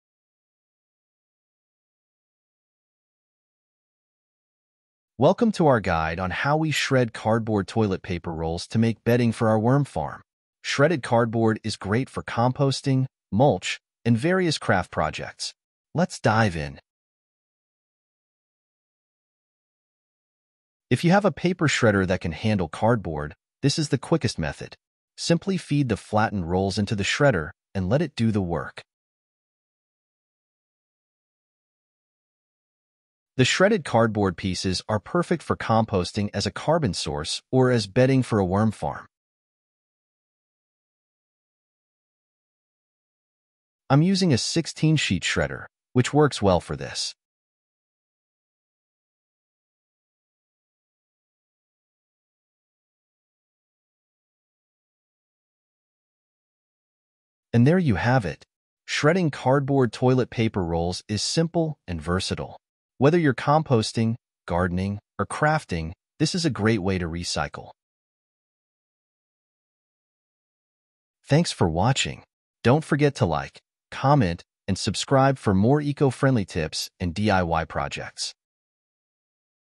Shredding Cardboard for Composting
Welcome to our easy guide on how we shred cardboard toilet paper rolls! 🧻 Whether you're looking to enhance your compost, create garden mulch, or embark on some fun craft projects, shredding cardboard rolls is a great way to recycle and repurpose.

In this video, we will walk you through shredding toilet paper rolls with a paper shredder.